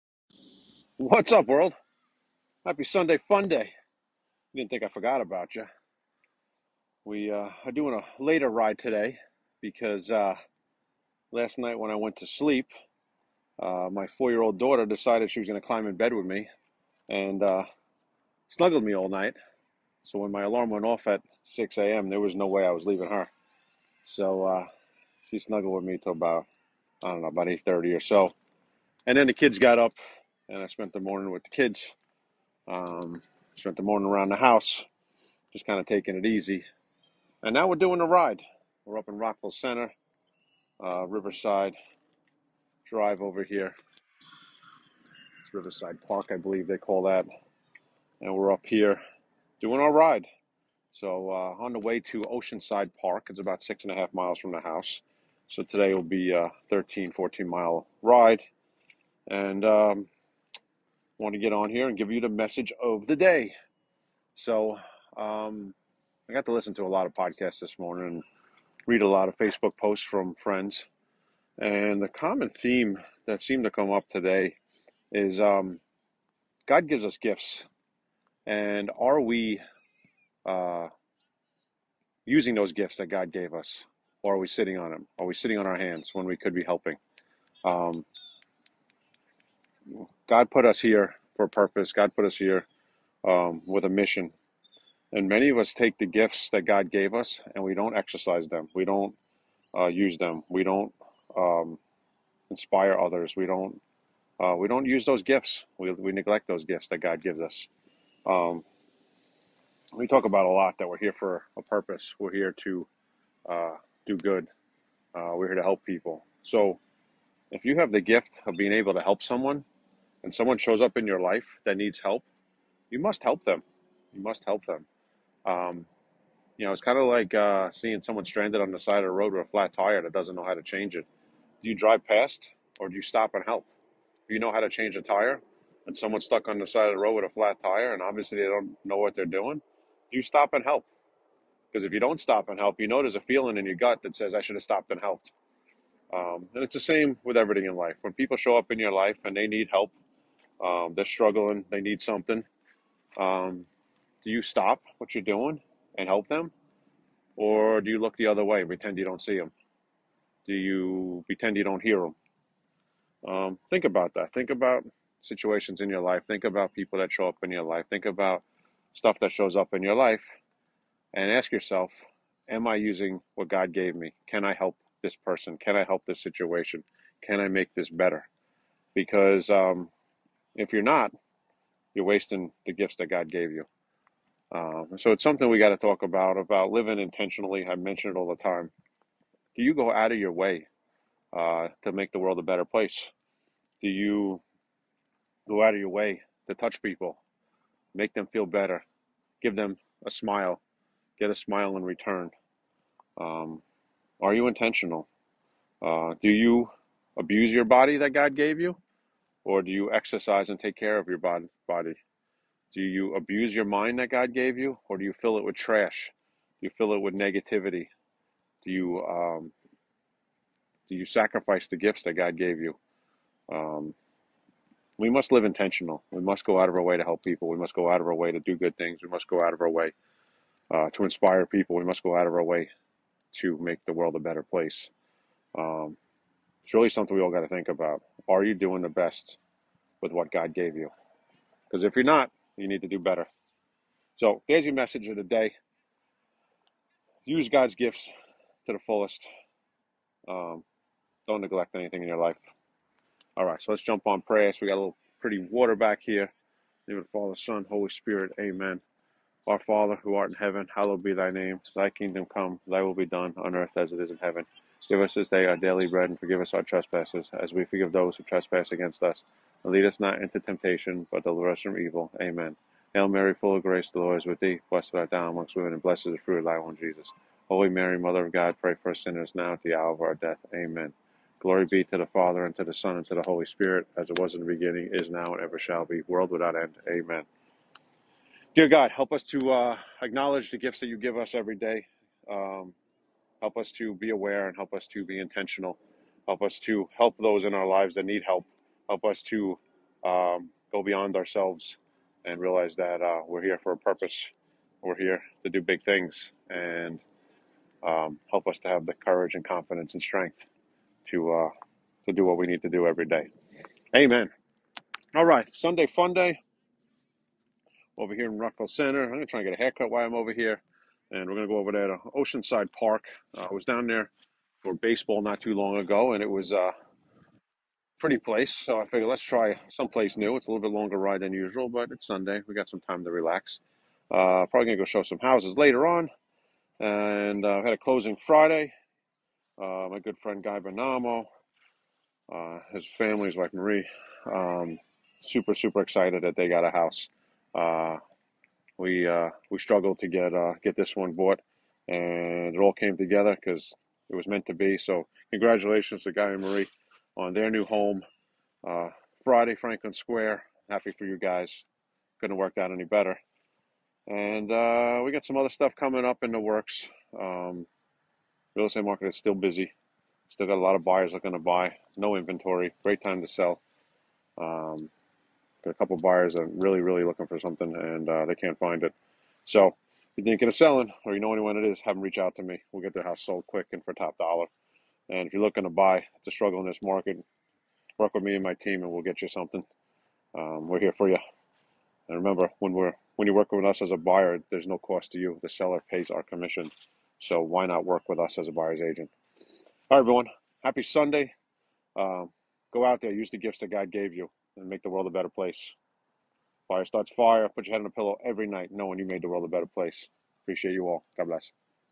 DAY 167: Rockville Centre, New York
Today's message:   Are we using the gifts that God gave us?